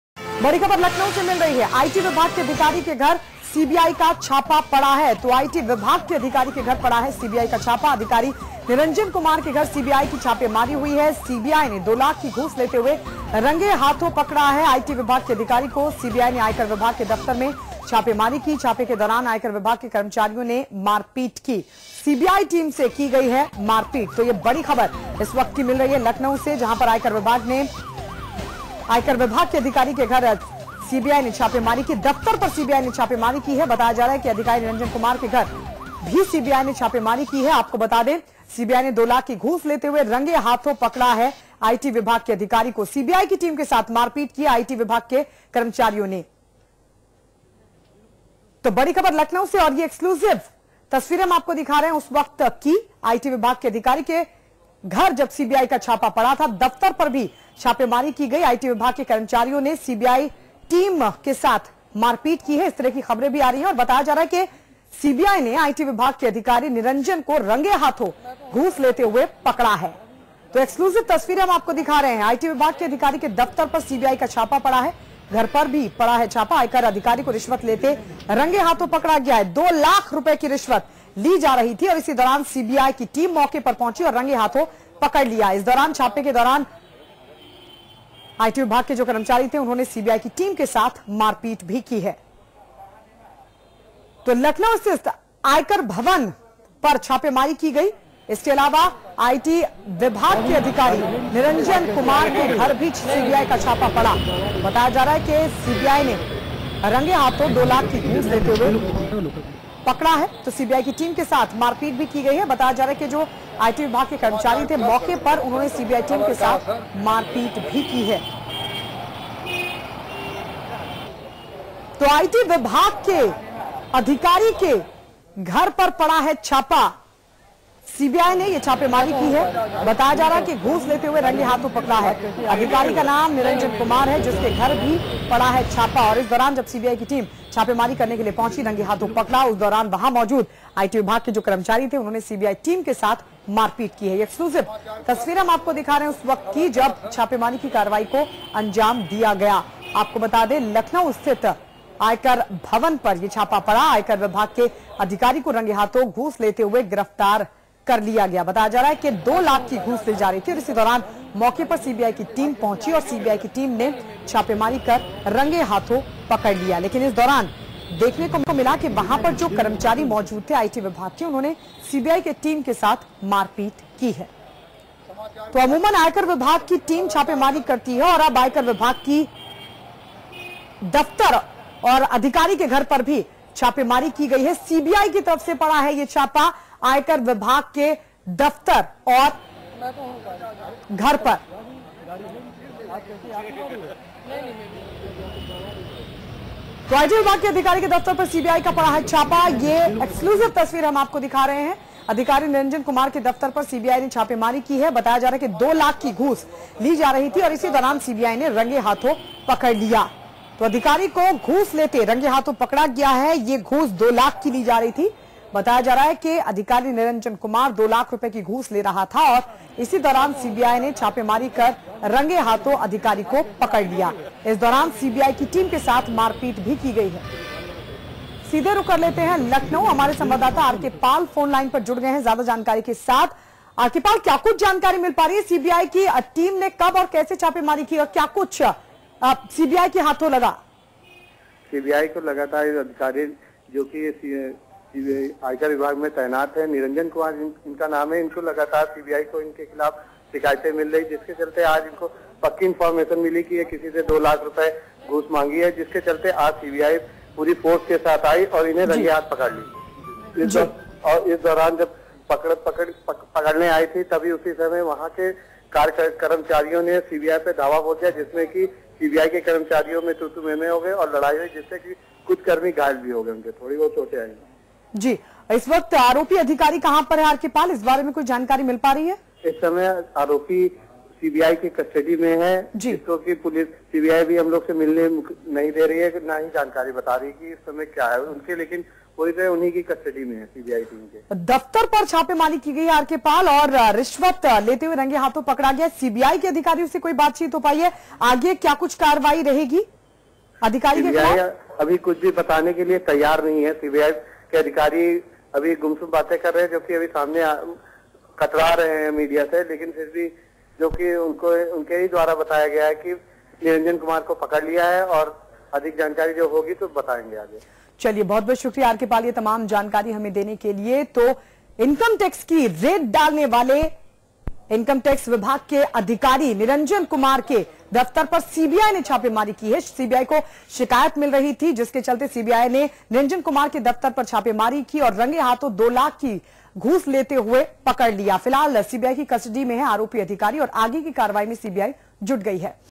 Income Tax Officer caught taking bribe; arrested by CBI
Income Tax Officer Niranjan Kumar was arrested by the CBI after he was caught taking Rs.2 lakh from a man who had complained to the CBI.